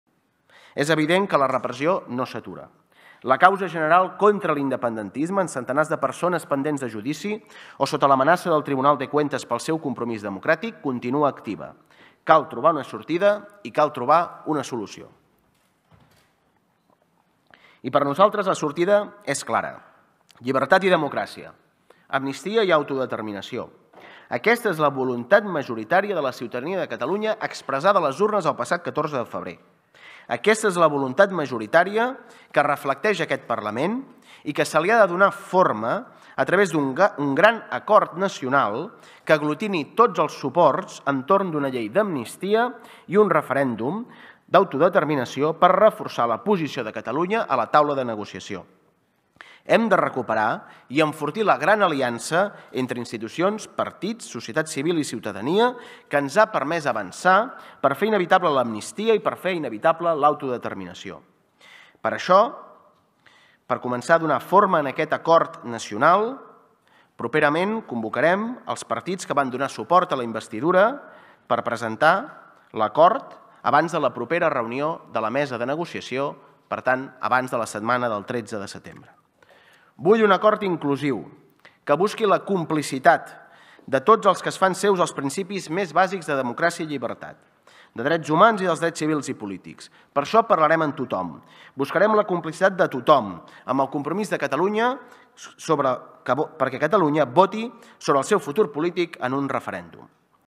Aragonès llama a una "gran alianza" por la amnistía y el referéndum
El presidente de la Generalitat, Pere Aragonès, ha llamado este miércoles a construir en las próximas semanas una "gran alianza" por la amnistía y un referéndum de autodeterminación para afrontar la mesa de diálogo con el Gobierno de Pedro Sánchez.

(Fuente: Parlament Catalunya)